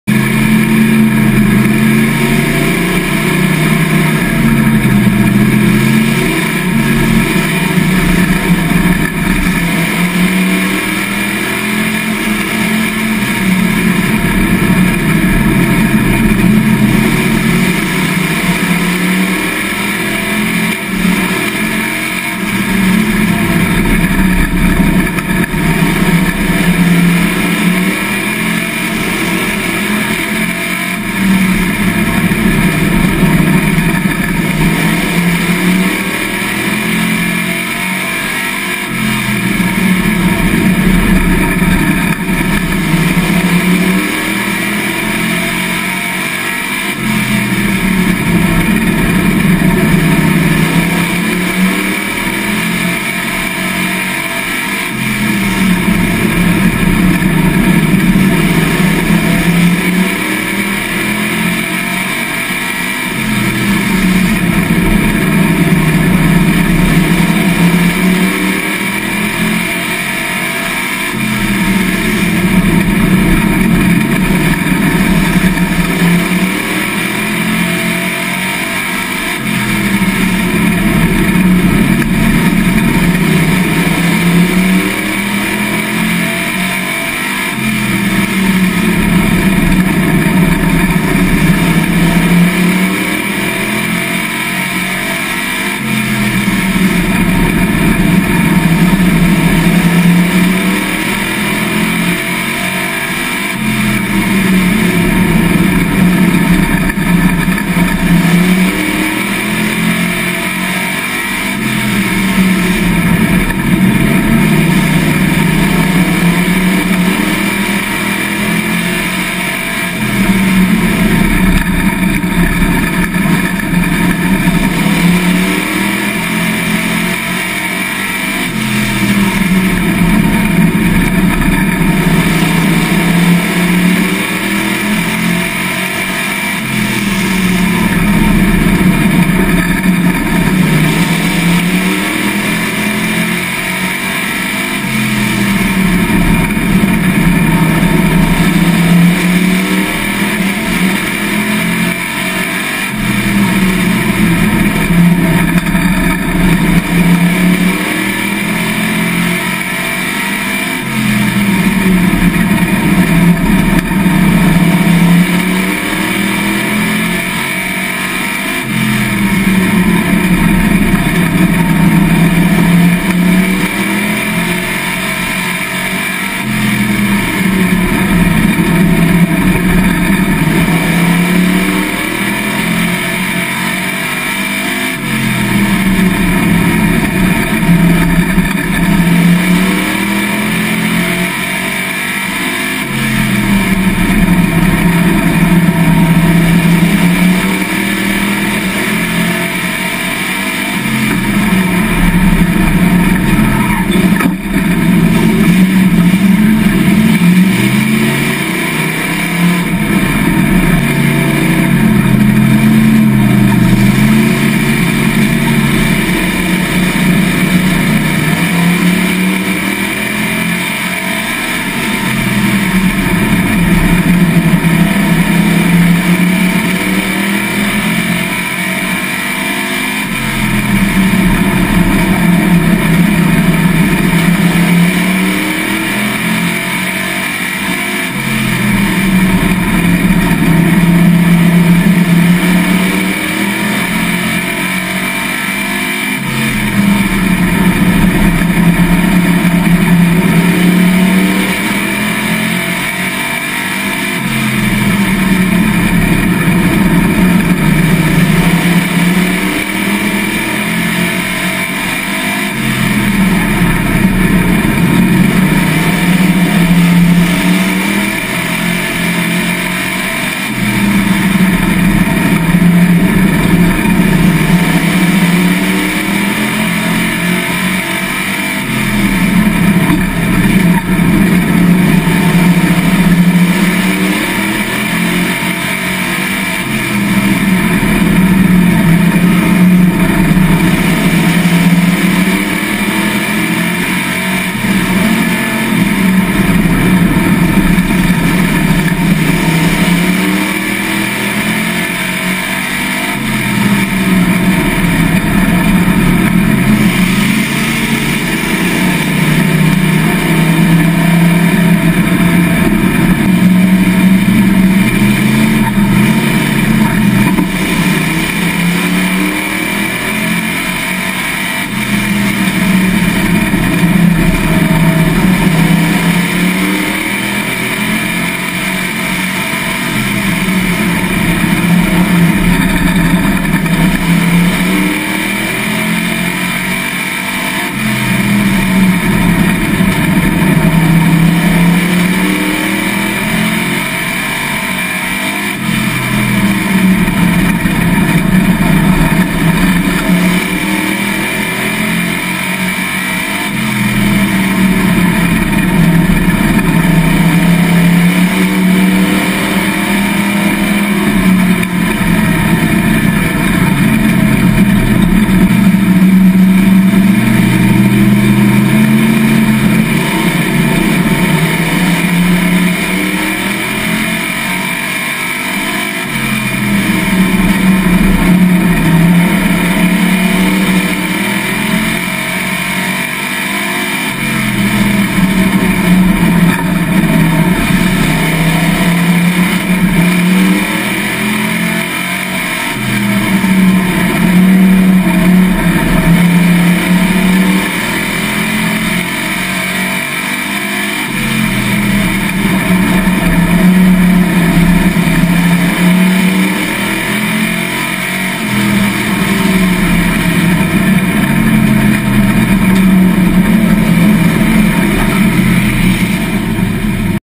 Whittemore Speedway May 26th, 2018 Leadsled Feature Race
K2 Kyle Edmonds - Plymouth Duster

Qualified 4th/16. Had a good race car and fought for the lead near the end before taking damage that ended the night for the Mopar.